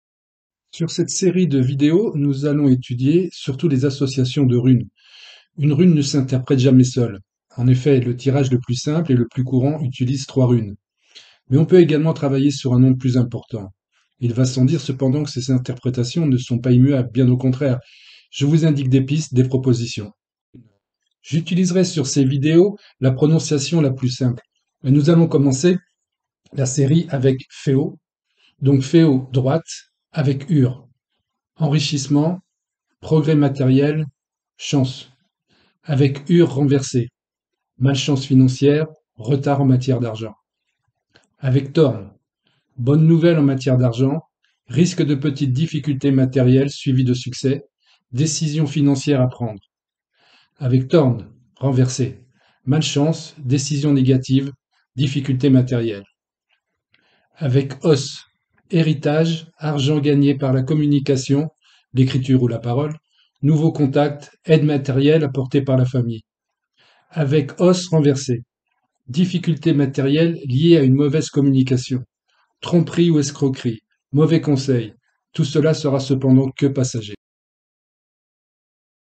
les runes en association
tuto sur les runes interprétation des runes droites renversées et en association à d'autres runes